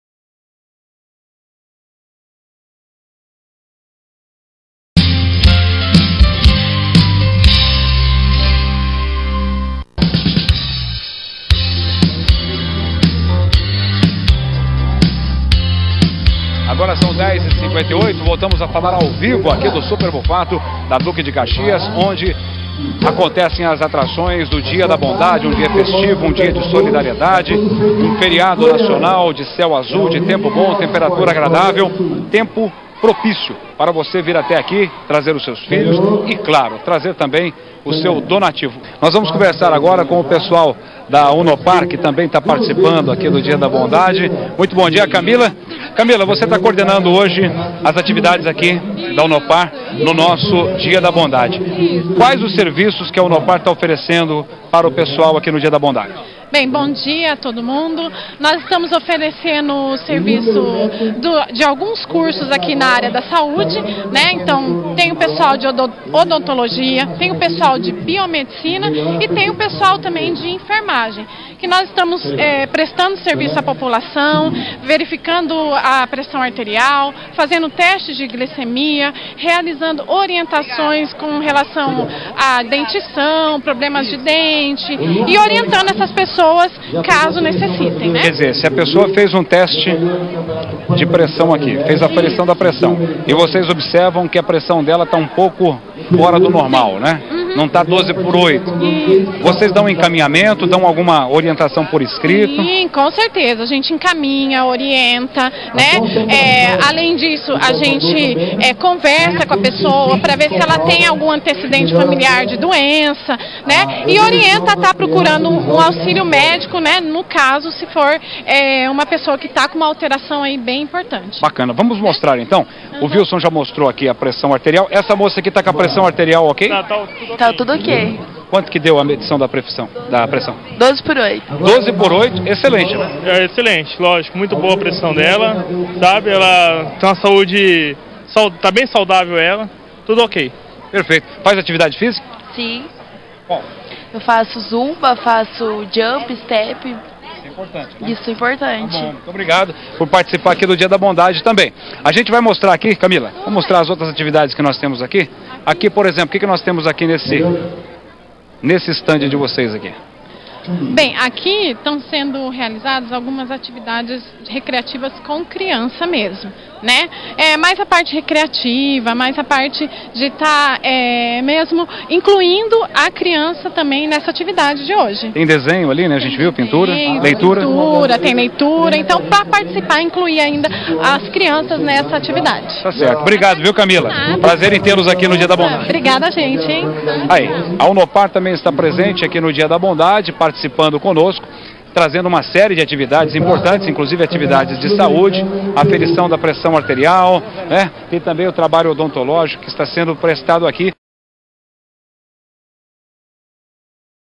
Unopar participa do Dia da Bondade
Unopar oferece serviços gratuitos à população no Dia da Bondade. Band/TV Tarobá. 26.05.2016.